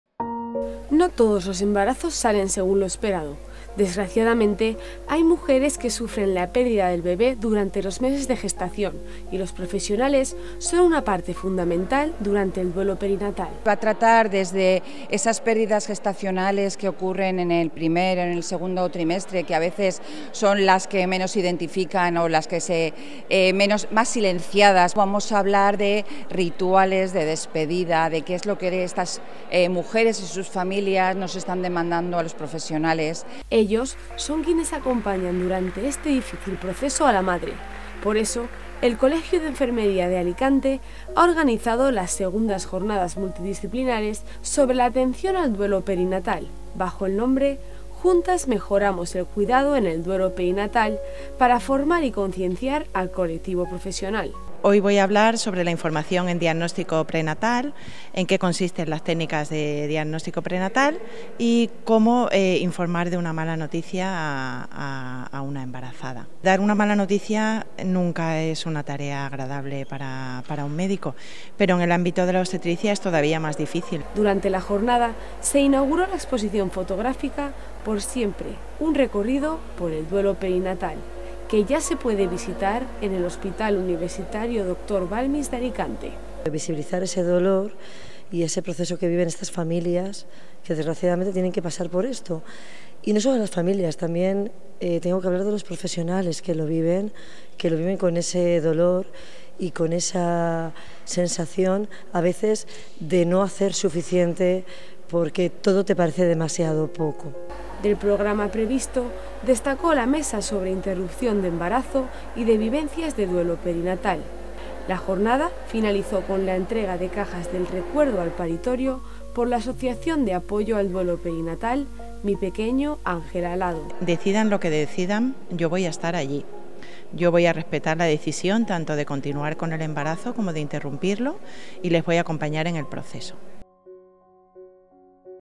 Las II Jornadas sobre el duelo perinatal visibilizan un tabú difícil para la sociedad | Cecova TV
No todos los embarazos salen según lo esperado y desgraciadamente, hay mujeres que sufren la pérdida del bebé durante los meses de gestación. Los profesionales acompañan durante este difícil proceso a la madre y por eso, el Colegio de Enfermería de Alicante ha organizado las «II Jornadas multidisciplinares sobre la atención al duelo perinatal» bajo el nombre «Juntas mejoramos el cuidado en el duelo perinatal» , para formar y concienciar al colectivo profesional.